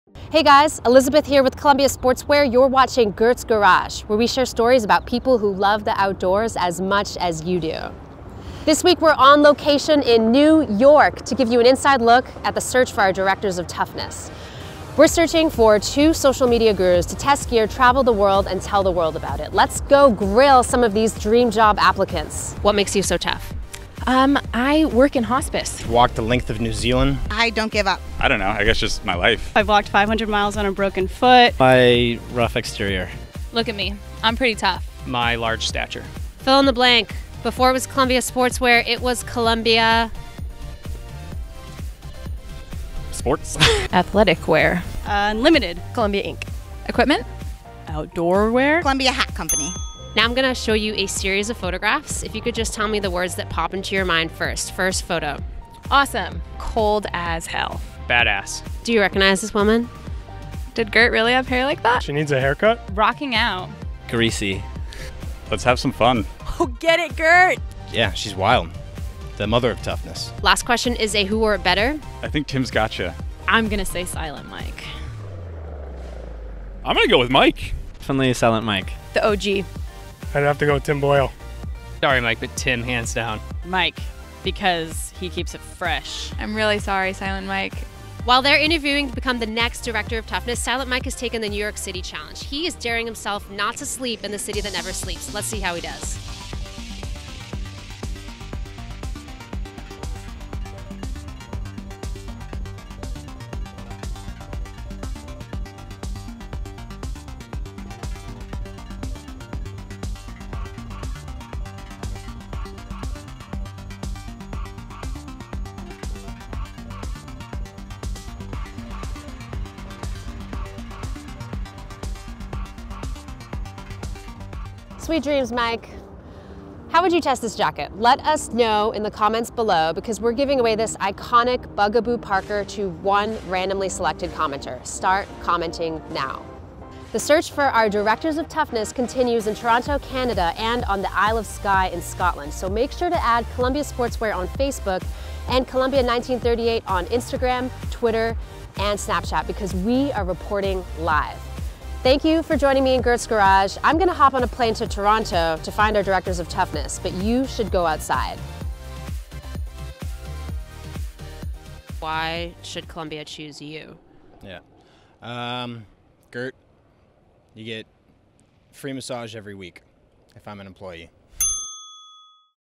Tested Tough In The City That Never Sleeps – Gert's Garage
This week on Gert's Garage we're giving you an inside look at the East Coast Director of Toughness interviews and Silent Mike takes the NYC 24 hour challenge.